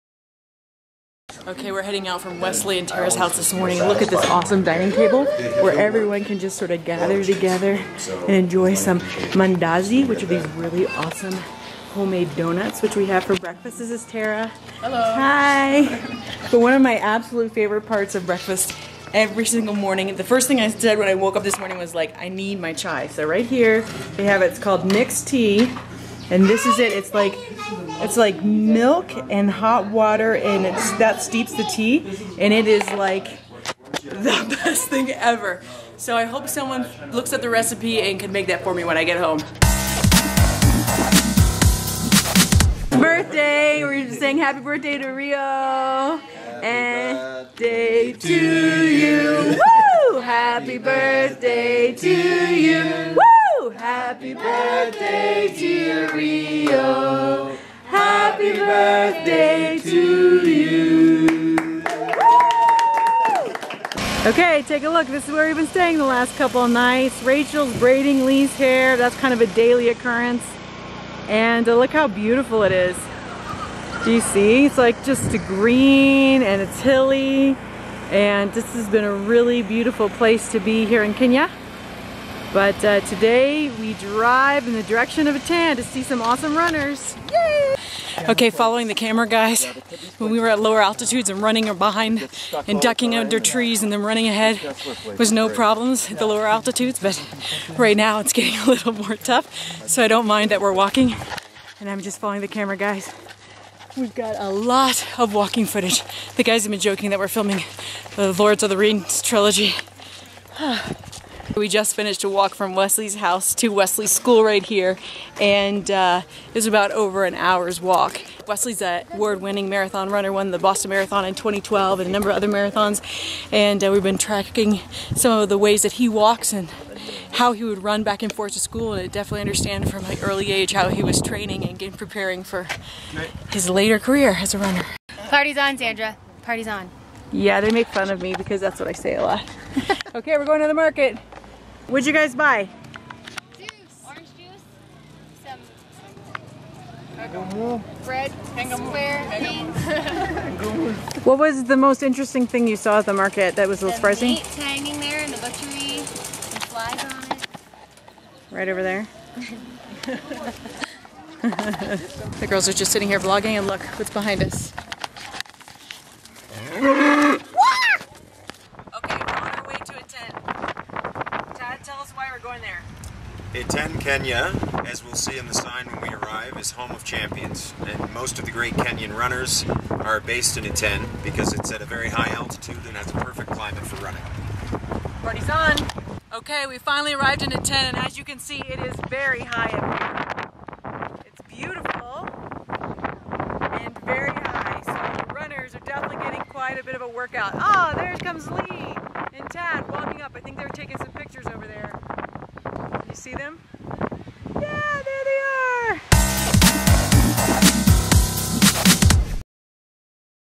Africa Trip - Day 13 (Birthday Breakfast, Market Shopping, Higher Altitudes)
The team celebrates a new friend's birthday and goes shopping before leaving for Iten which is home to many of Kenya's top long distance runners.